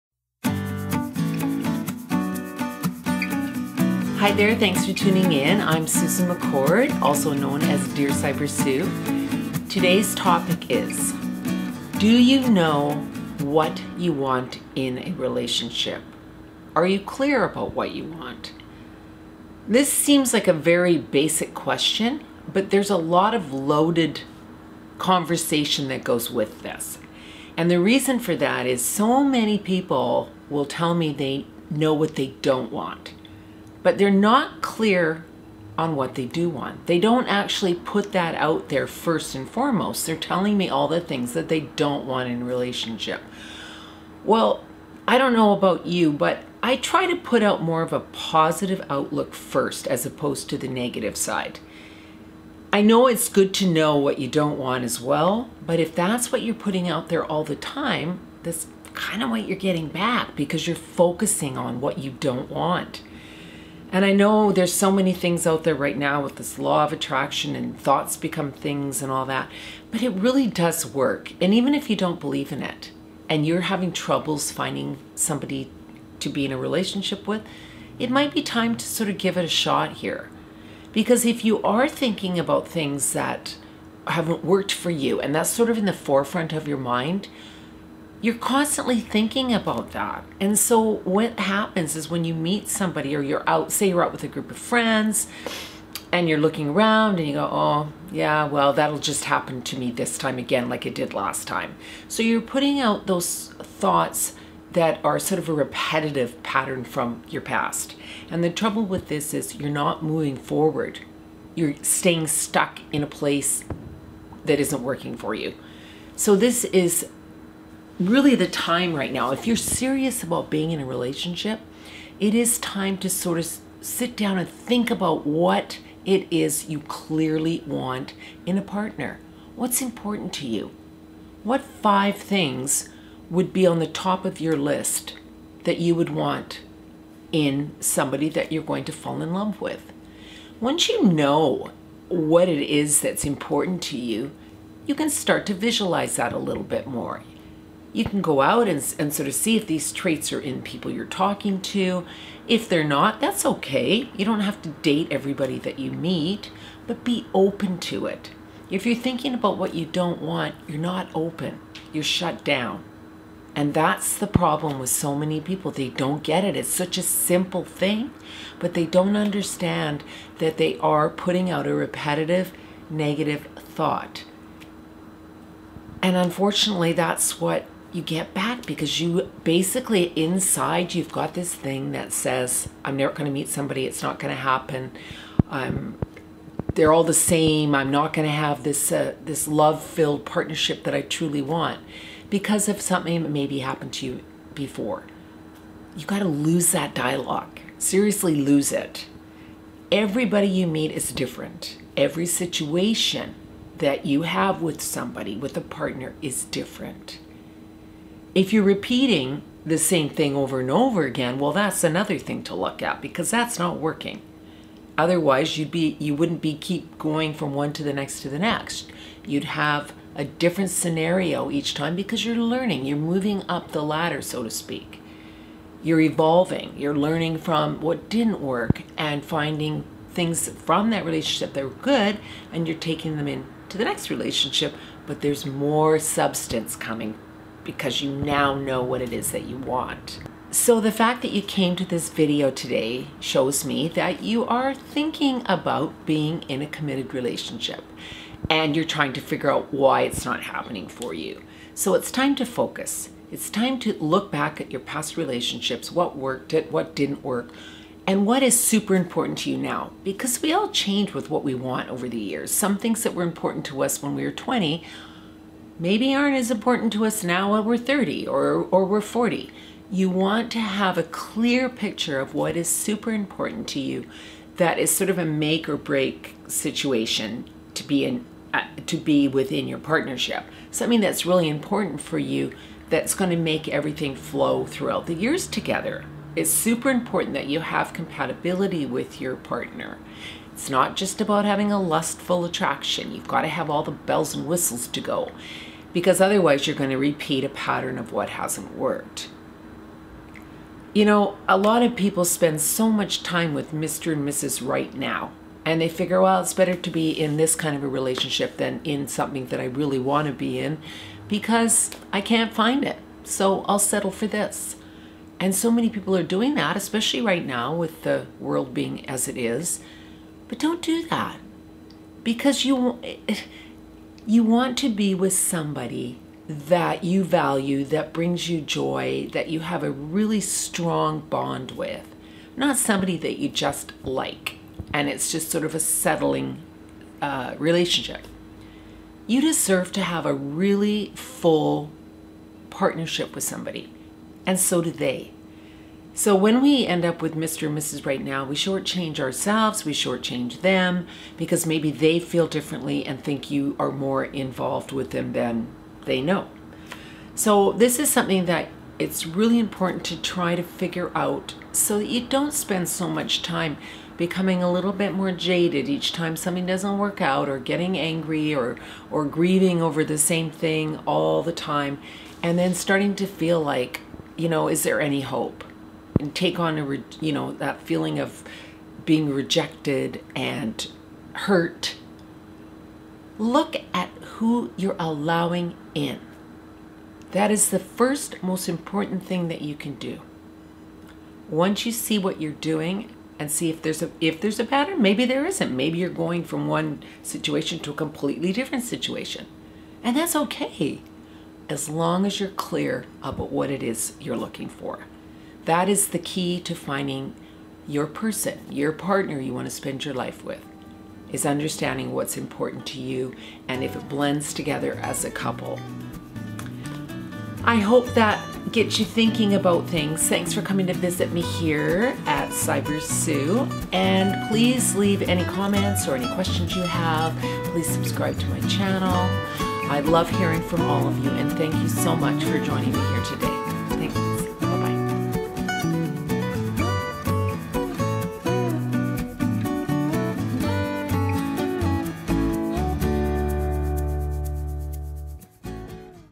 Are You Clear About What You Really Want in a Relationship?
Are You Clear About What You Really Want in a Relationship? Are you frustrated that you are not meeting that special person to share your life with? Dating/relationship coach and advice columnist Dear Sybersue wants you to pay close attention to understanding what it is that you truly want in a partnership. Many people "know what they don't want" but they are not clear on what they do want, and what is most important to them.

If you are focusing your energy on hurtful memories from your past relationships you will continue to attract similar people towards you. This internal dialogue can keep you stuck for a long time without you even being aware that these thoughts are holding you back from finding love. It's time to change up your thought process and recharge your life so that you can be in a happy partnership.

Private Consultation Dating or Relationship Questions for Sybersue? Thank you!